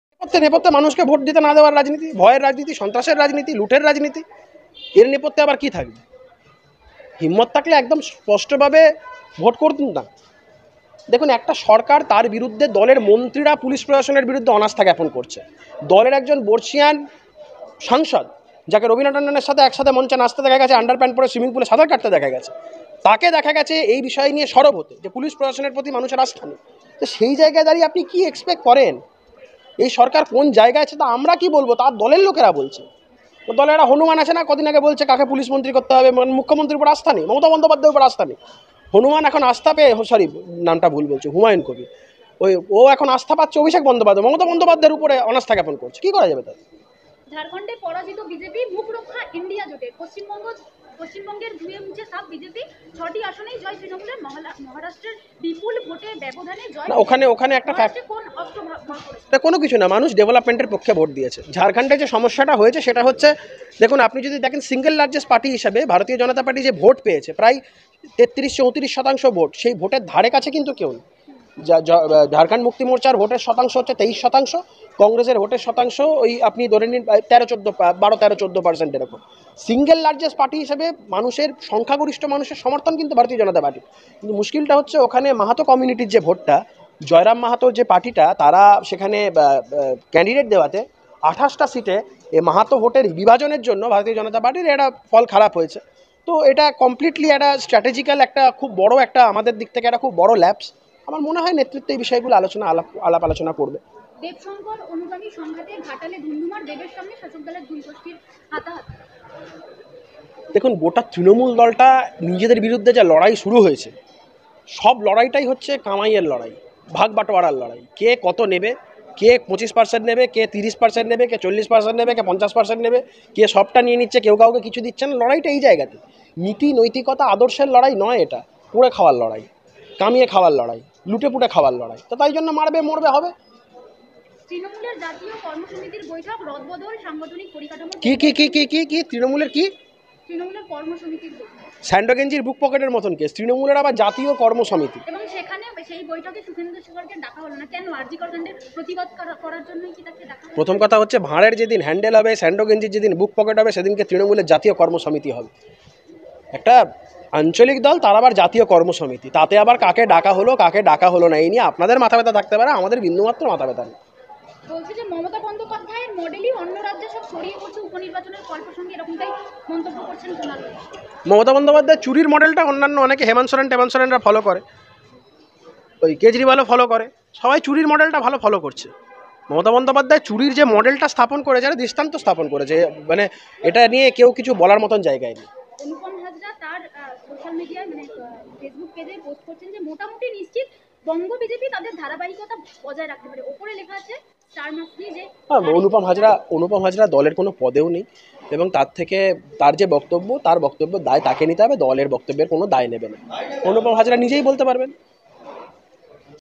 'হিম্মত থাকলে স্পষ্টভাবে ভোট করুন..' কটাক্ষ আইনজীবী কৌস্তভ বাগচীর
Koustav Bagchi : 'হিম্মত থাকলে স্পষ্টভাবে ভোট করুন..' কটাক্ষ আইনজীবী কৌস্তভ বাগচীর
Bengal News
bengal politics
bengal politics news
Bengali News
Bibidho News